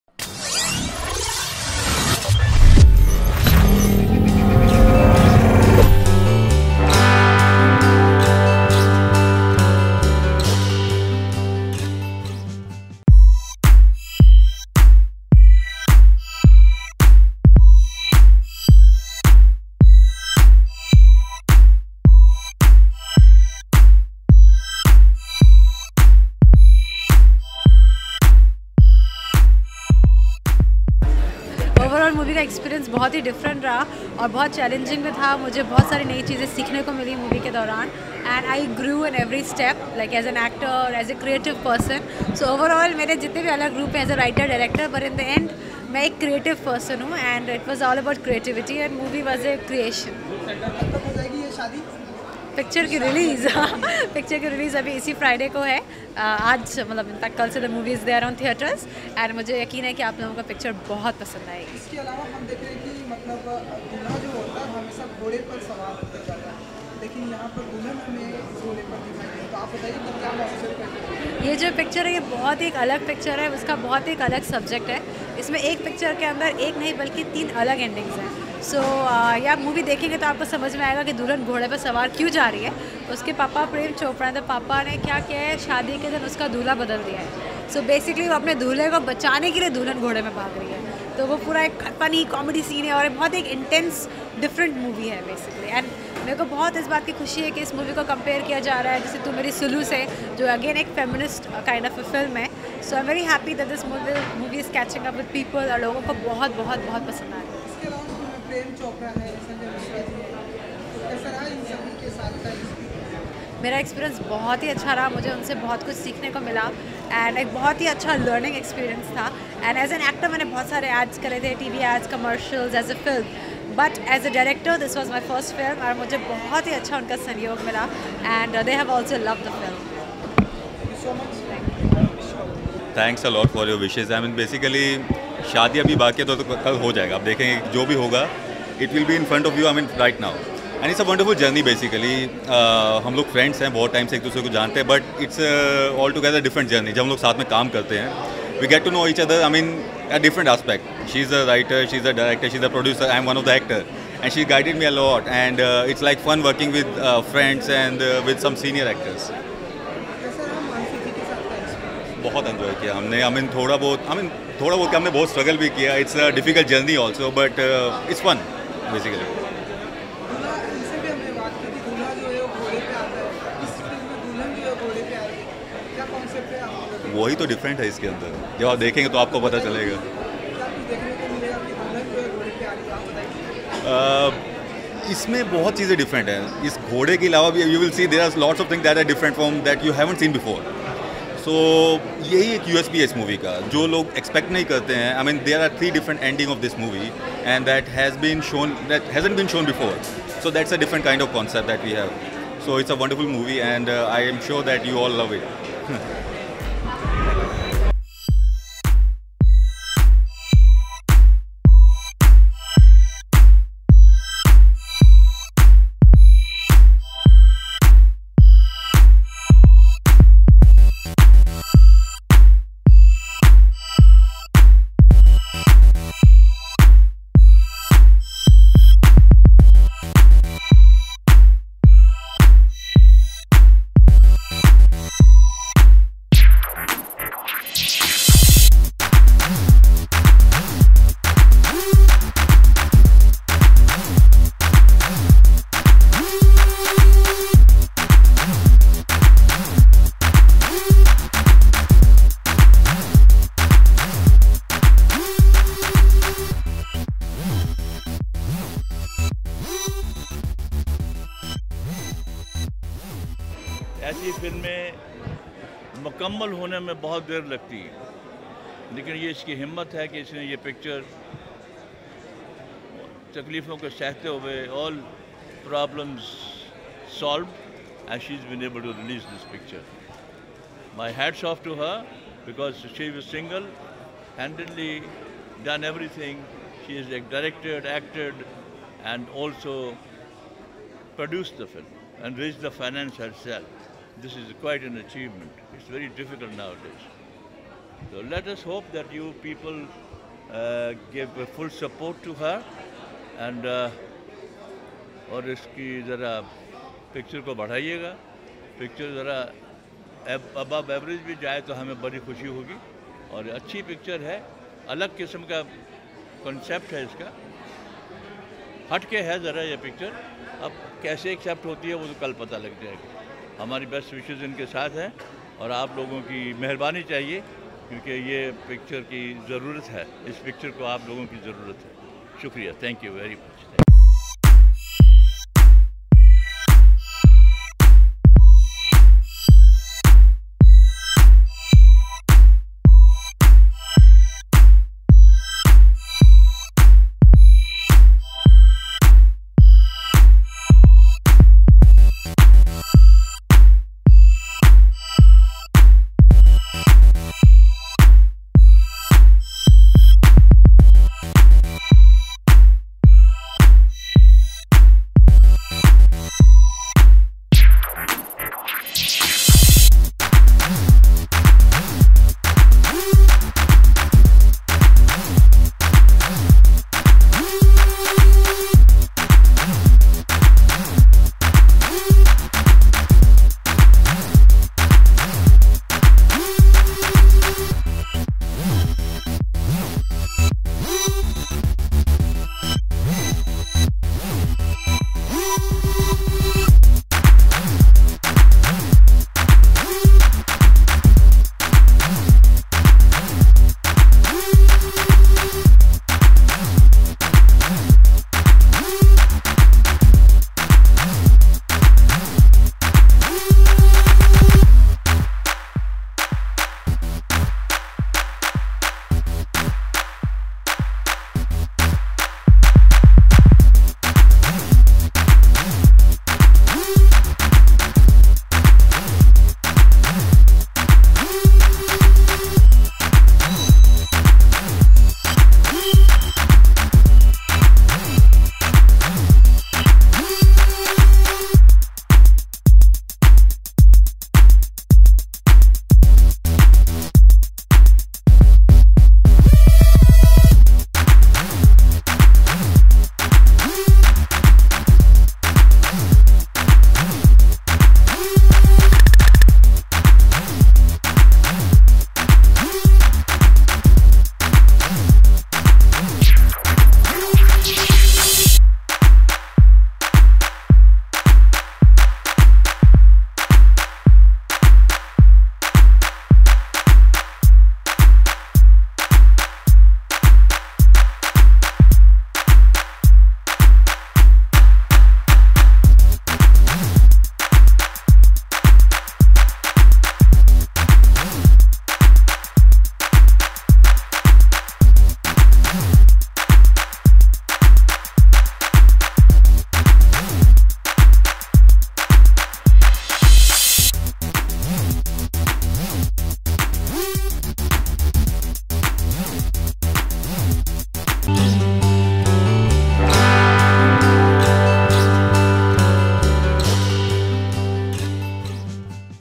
Prem Chopra की Film Shaadi Abhi Baaki Hai की रखी गई Special Screening
Mansi Dovhal's directorial debut 'Shaadi Abhi Baaki Hai' starring Sanjay Mishra in a completely new avatar and the legendary Prem Chopra is all set to hit theatres on November 17, 2017. recenlty stars attended the screening of this film.To know more watch this video